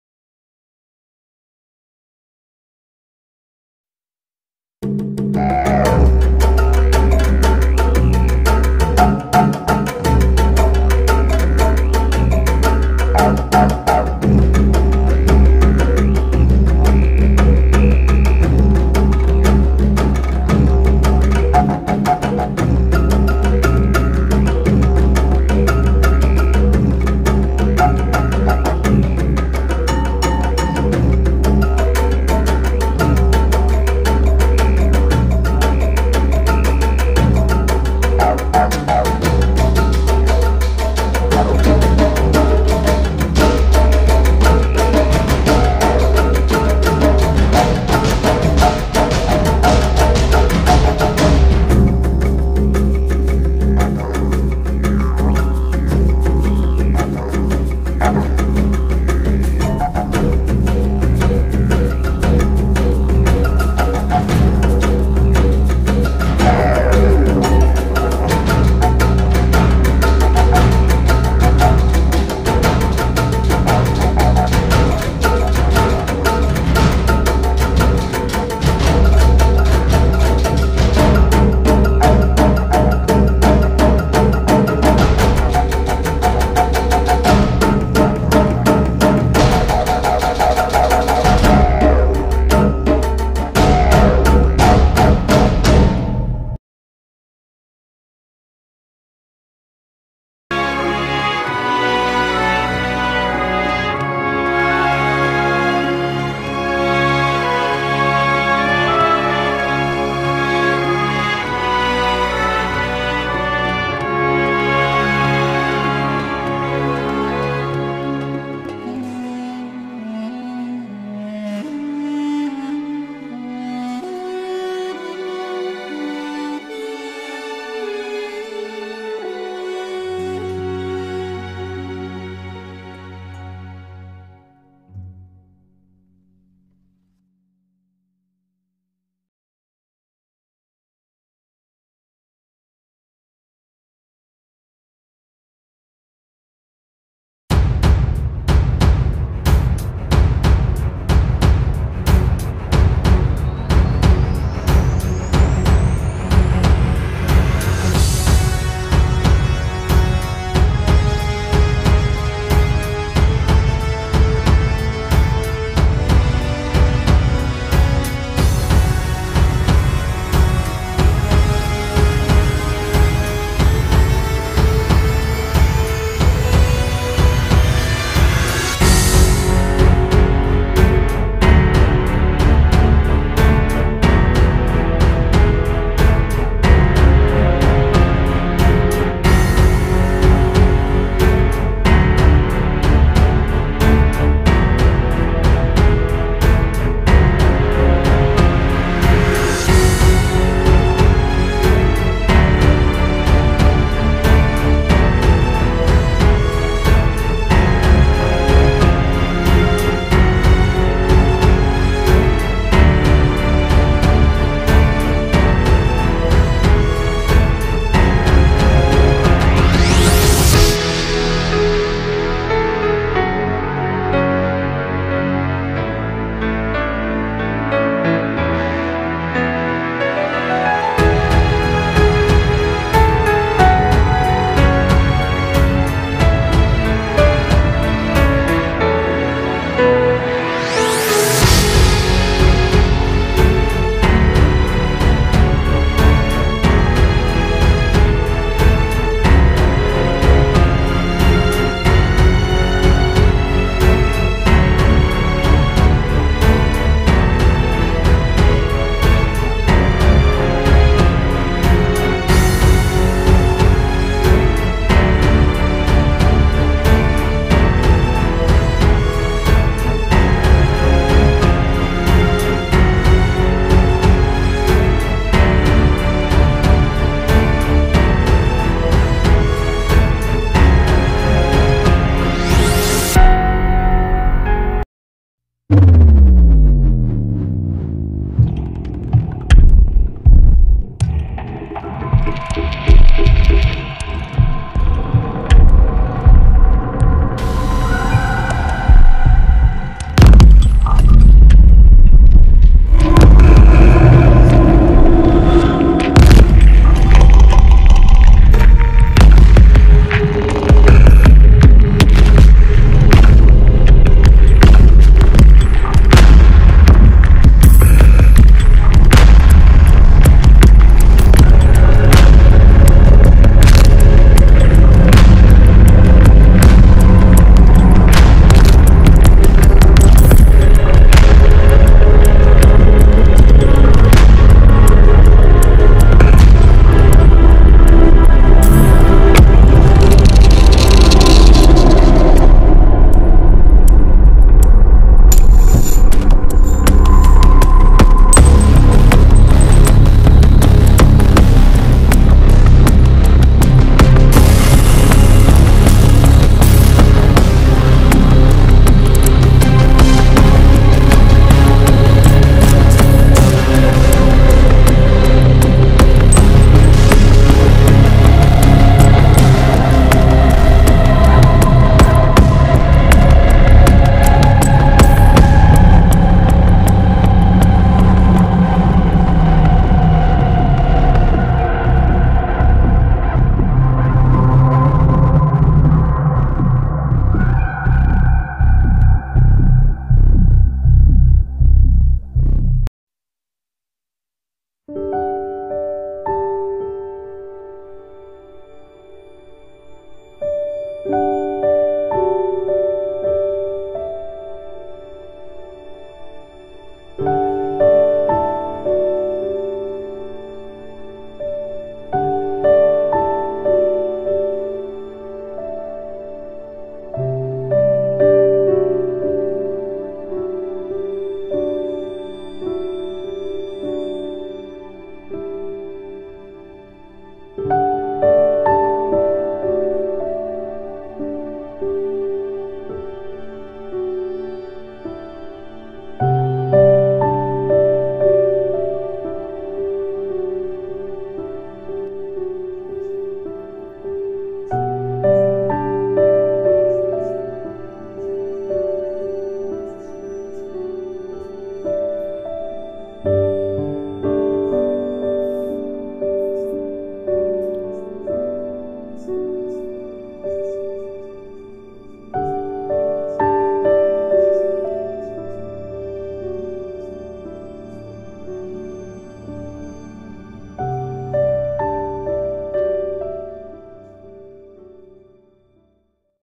Аквачелендж буква "Л" от блогера Vari Live лиман в аквариуме
Нарезка фото и видео лимана в Николаевской области и животных которые живут в аквариуме из лимана